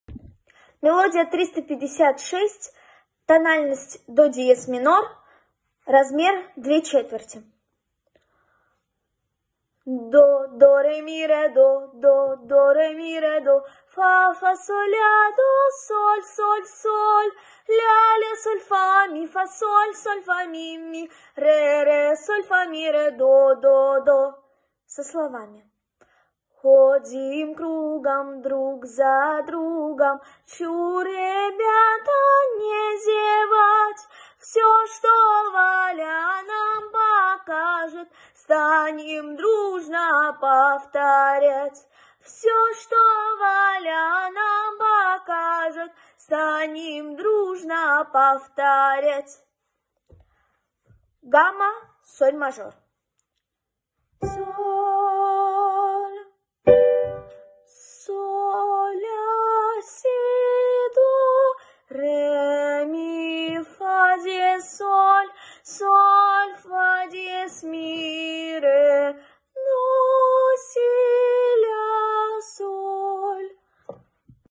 Пономарева Софья-4класс ДОП ,преп. Барсегьянц Л. С.
Пономарева  Софья-4класс ДОП ,преп. Барсегьянц Л. С.Подготовка к итоговой аттестации -работа  над пением гаммы и мелодии.